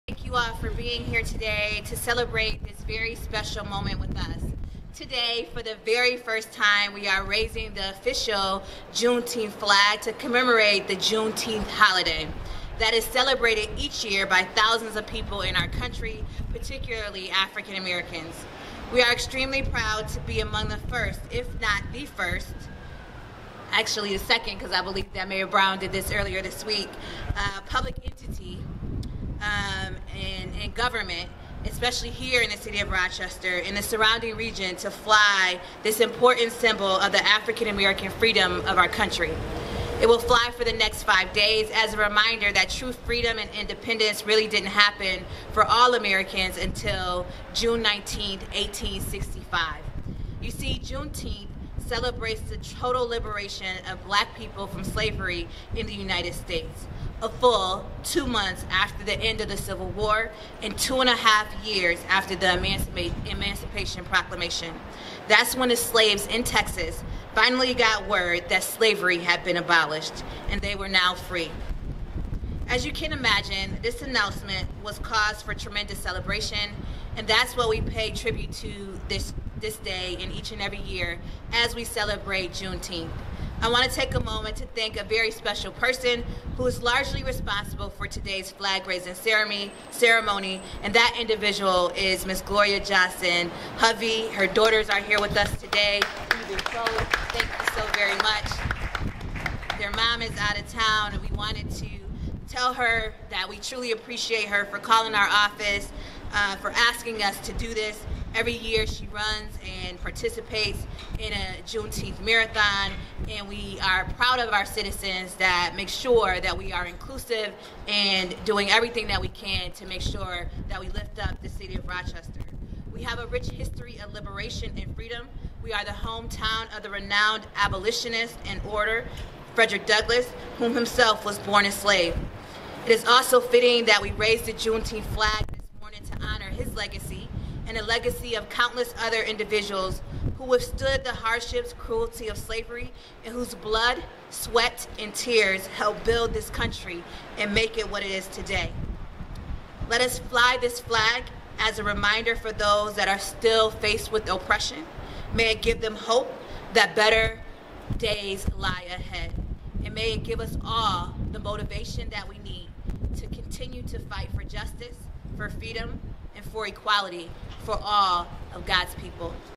Mayor Lovely A Warren ( official Juneteenth flag celebration)
Mayor Lovely A. Warren raised the official Juneteenth flag at
City Hall to commemorate the Juneteenth holiday.